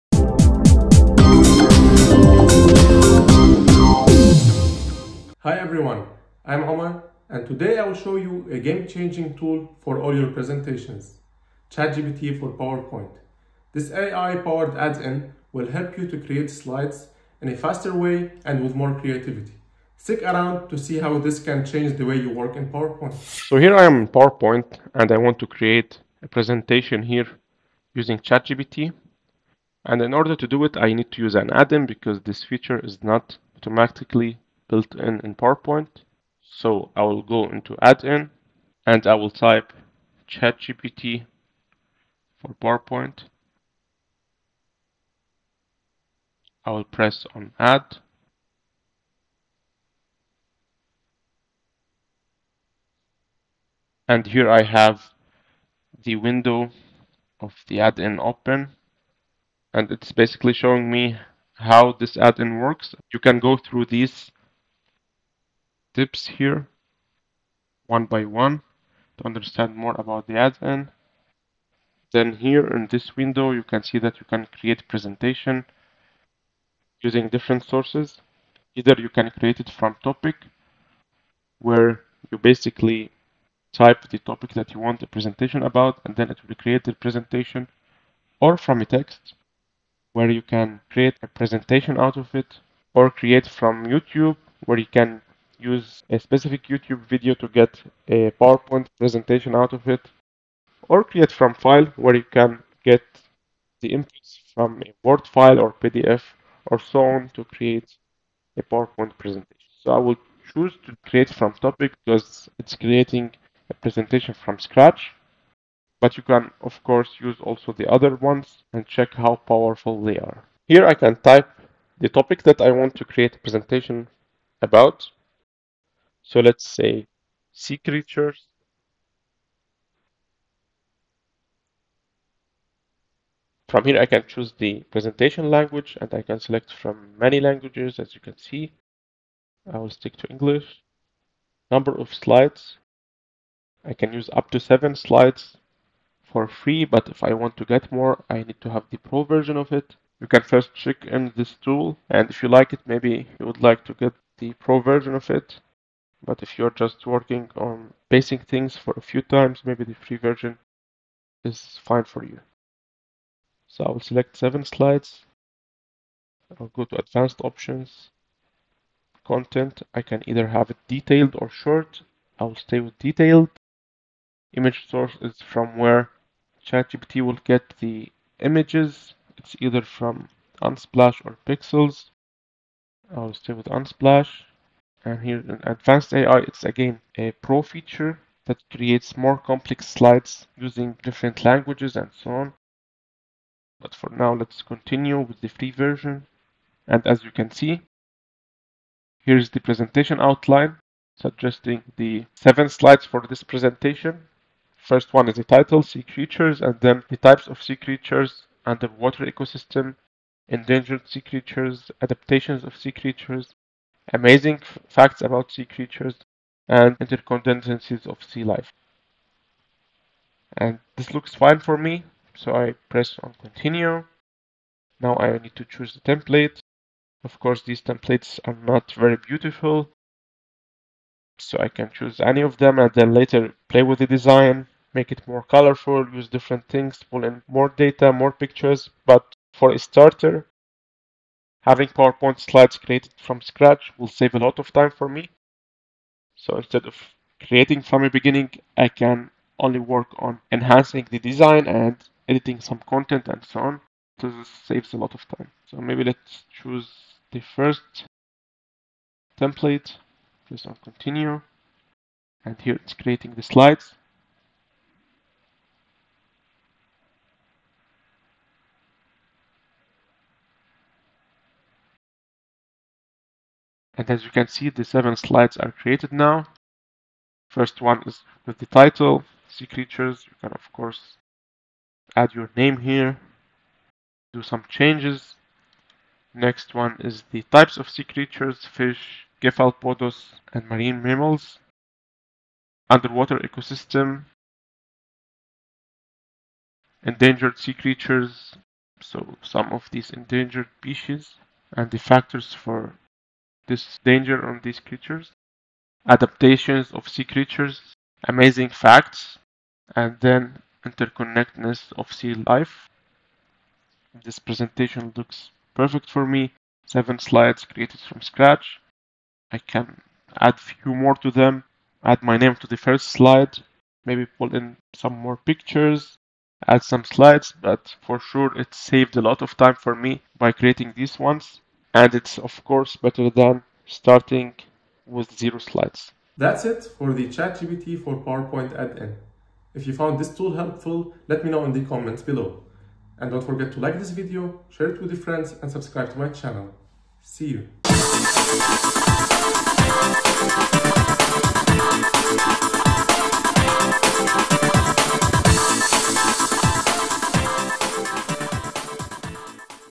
How to Use ChatGPT in PowerPoint: Boost Your Presentation Skills with This Game-Changing Add-In!
Ready to take your PowerPoint presentations to the next level? Discover how to integrate ChatGPT with PowerPoint using this powerful add-in! In this tutorial, we'll walk you through the setup, features, and best practices for using ChatGPT to generate slide content, enhance creativity, and streamline your workflow.
Whether you're a student, professional, or educator, this tool is a must-have for crafting impressive and engaging presentations in minutes.